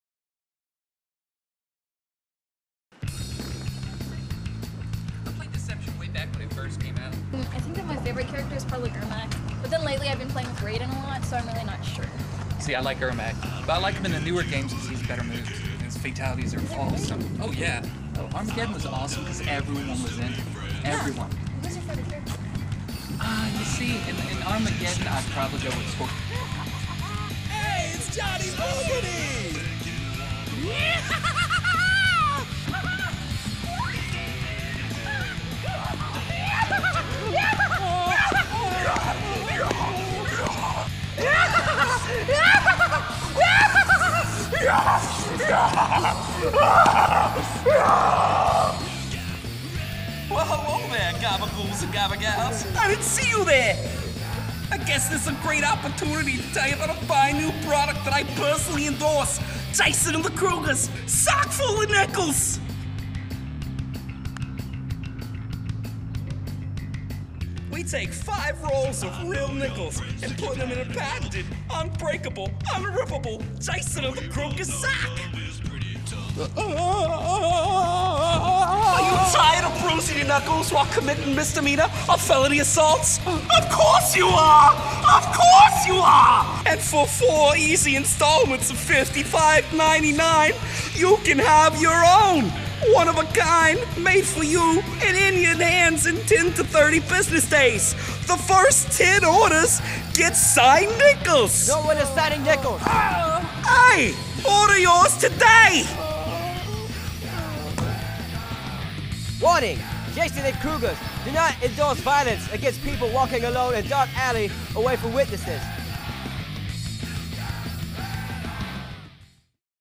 SOCK FULL OF NICKELS
Jason and the Kruegers have a new product to help you prepare for the future.